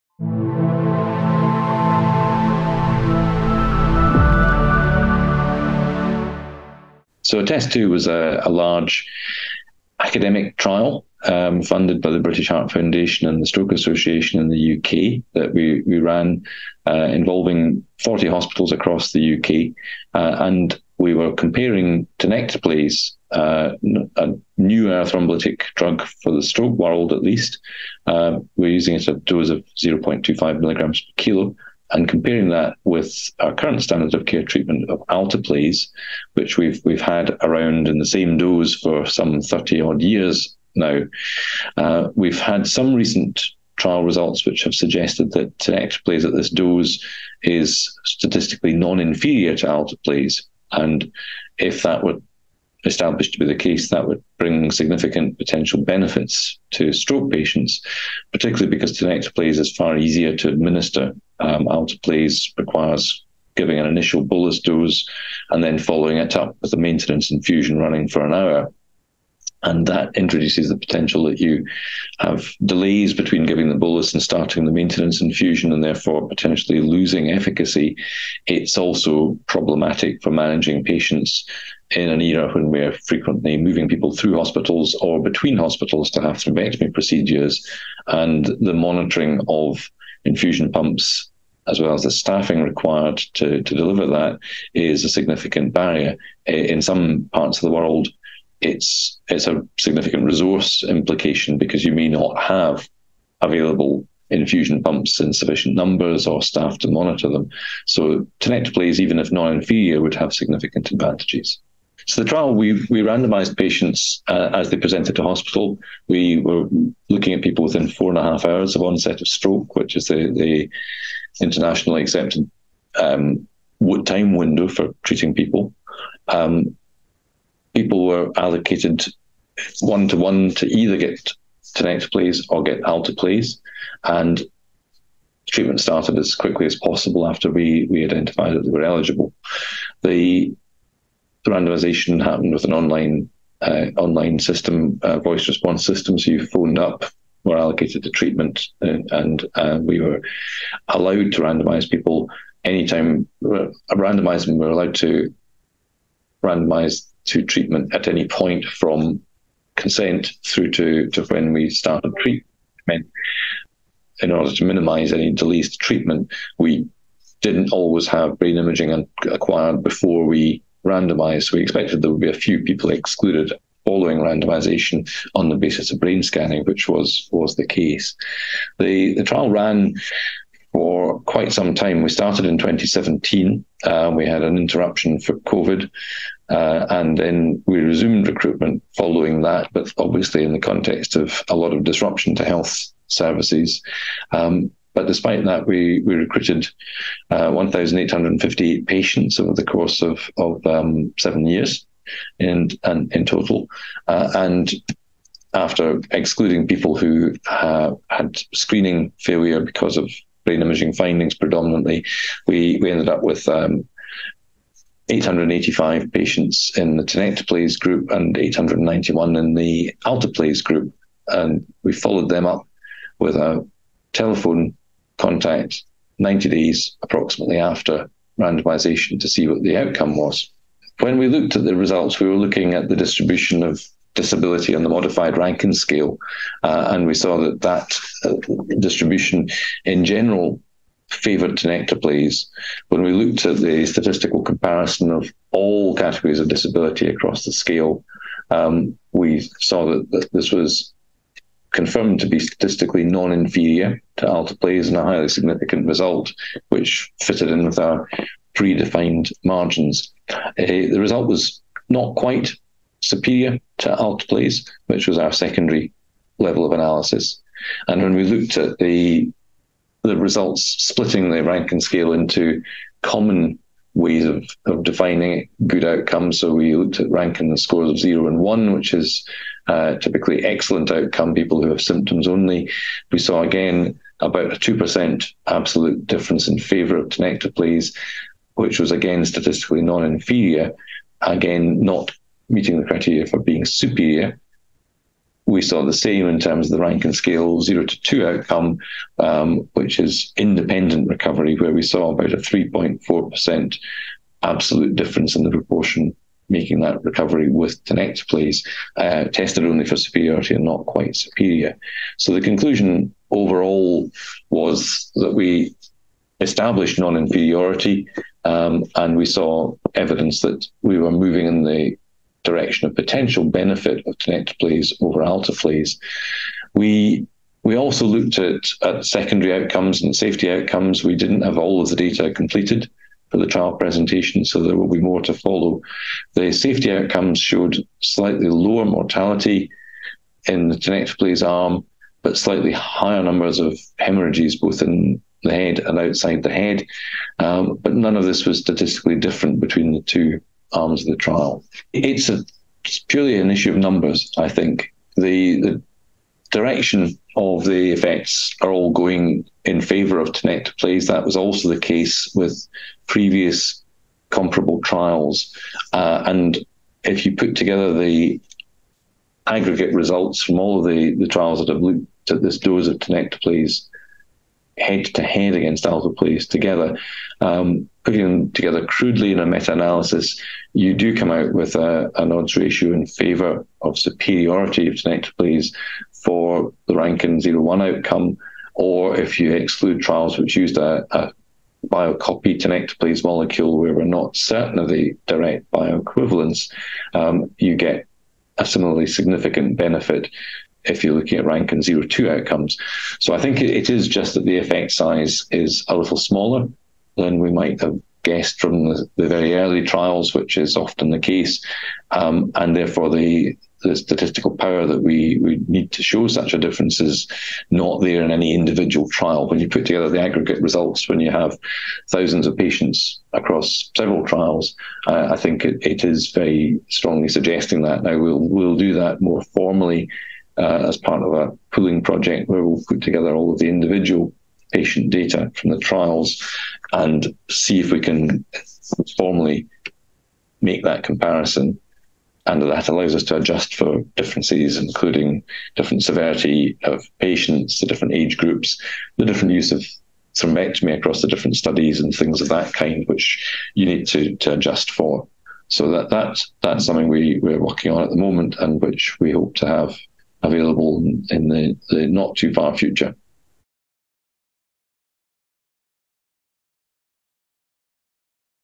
ATTEST2: alteplase versus tenecteplase for stroke thrombolysis
Keith Muir, MD, PhD, Queen Elizabeth University Hospital, Glasgow, UK, gives an update on the multicenter ATTEST2 trial (NCT02814409) comparing thrombolytic agents, alteplase versus tenecteplase, for reperfusion therapy after acute ischemic stroke. Over the course of seven years, this trial enrolled over 1,800 patients, randomly assigning them to receive either alteplase or tenecteplase treatment within 4.5 hours of stroke symptom onset. After a 90-day period, patients were assessed on their level of disability using the modified Rankin scale. Tenecteplase demonstrated non-inferiority to the more commonly used alteplase. In line with several other trials, this finding may further encourage a shift in clinical practice, favoring tenecteplase due to its ease of administration. The study did not, however, establish the superiority of tenecteplase. Prof. Muir attributes this to a lack of statistical power within a single study to reveal the subtle advantage of tenecteplase. However, he suggests that if the results of numerous studies comparing alteplase and tenecteplase were combined in a meta-analysis, the superiority of tenecteplase may become clear. This interview took place at the World Stroke Congress (WSC) 2023 in Toronto, Canada.